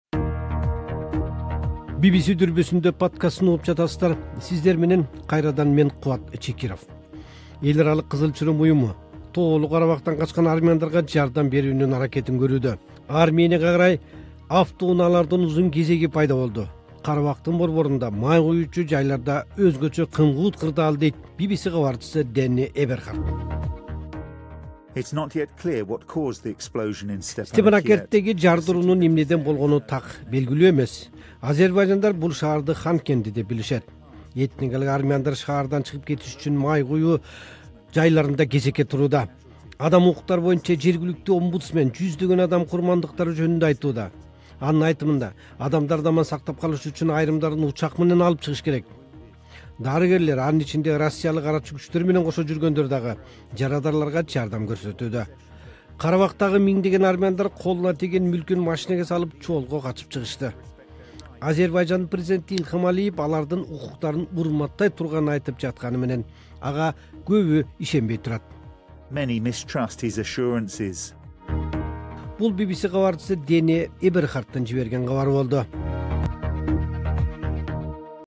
Тоолуу Карабактан качкан армяндар - подкаст BBC Kyrgyz
Эл аралык кызыл чырым уюму Тоолуу Карабактан качкан армяндарга жардам берүүнүн аракетин көрүүдө. Арменияга карай автоунаалардын узун кезеги пайда болду. Карабактын борборунда май куючу жайда кымгуут кырдаал, дейт Би-Би-Си кабарчысы Дэнни Эберхард.